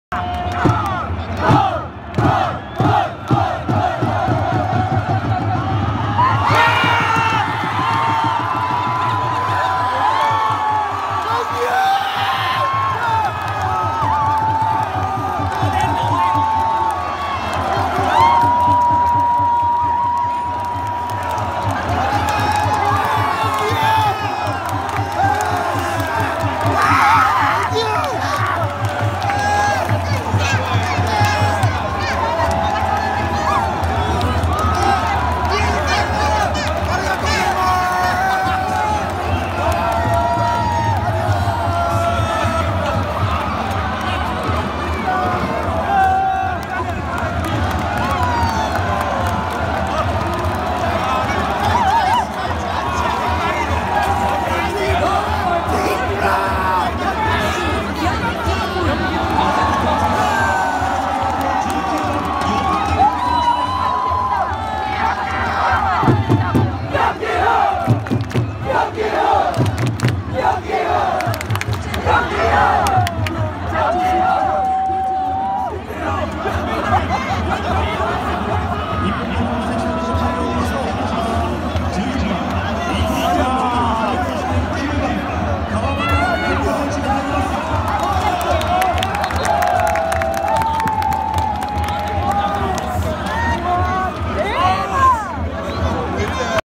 日本対韓国 ヨム・ギフン選手ゴール 後半 9/11
2017年12月16日(土) 19時15分
EAFF E-1フットボールチャンピオンシップ2017
일본 vs 대한민국
Japan vs Korea Republic
味の素スタジアム 36,645人
3分 小林悠選手ゴール
13分 キム・シヌク選手ゴール
23分 チョン・ウヨン選手ゴール
35分 キム・シヌク選手ゴール
69分 ヨム・ギフン選手ゴール
1-4